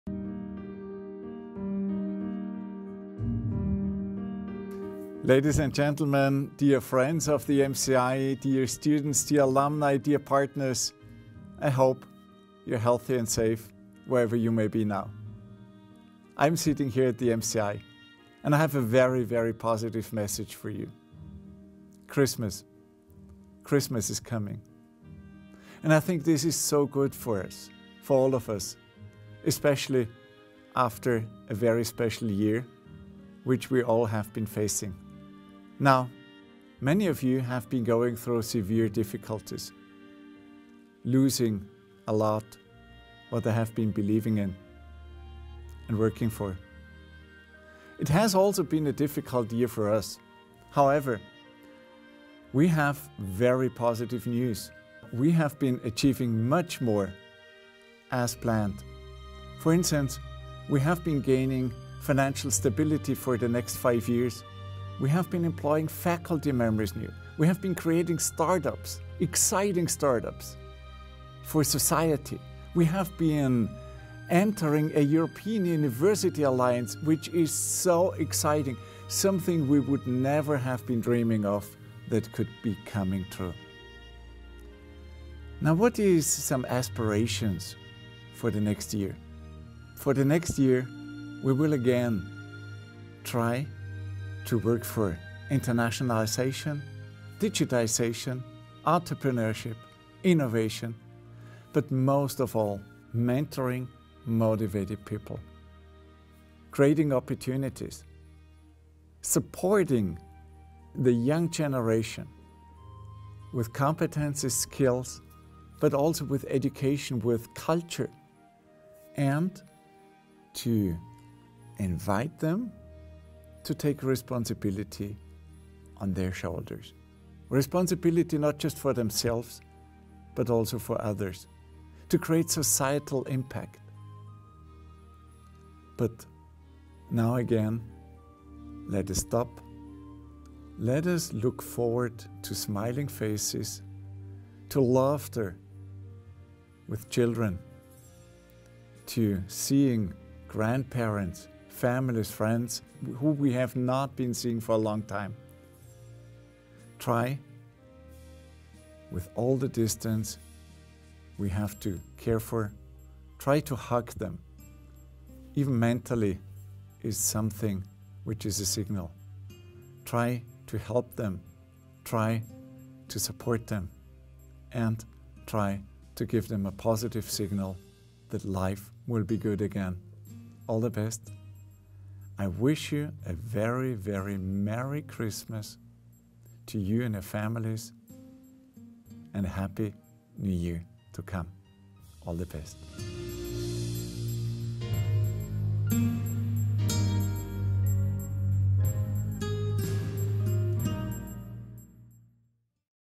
XMAS 2020 | Greetings from Rector Andreas Altmann | MCI The Entrepreneurial School®
Rector Andreas Altmann is greeting MCI students, alumni, faculty, partners and friends.